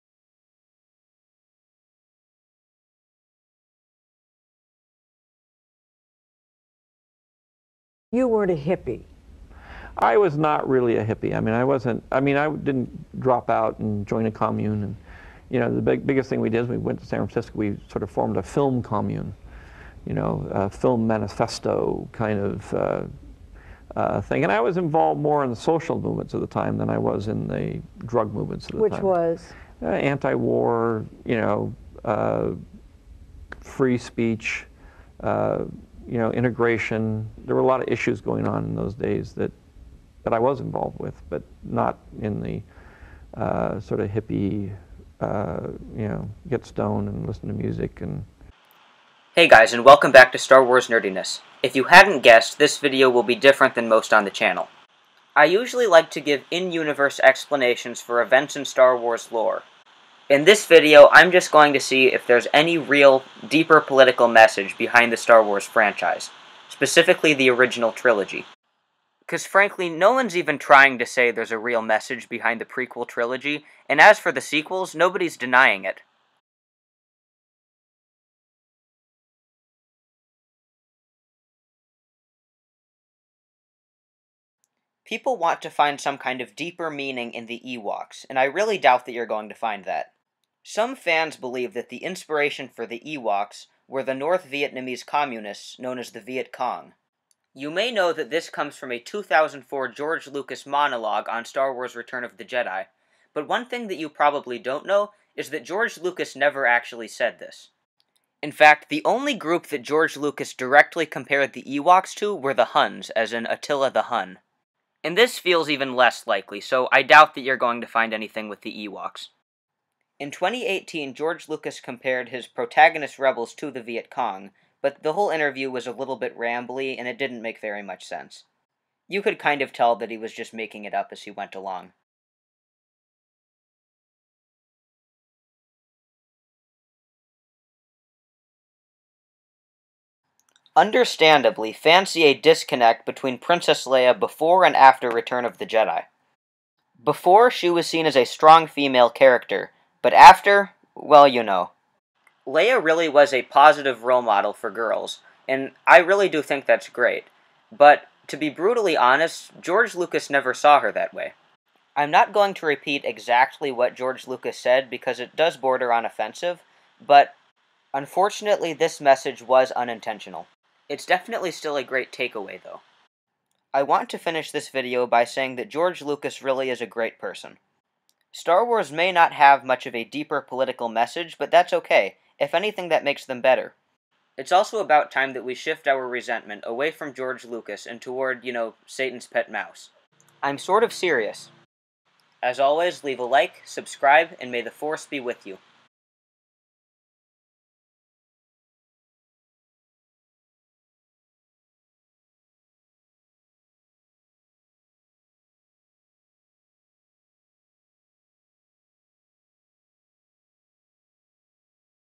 The Political Message Behind STAR WARS
Is there really an underlying message behind the STAR WARS saga, or was it just made for entertainment? In this video, we will explore the different theories, and I will give my own opinion on this question.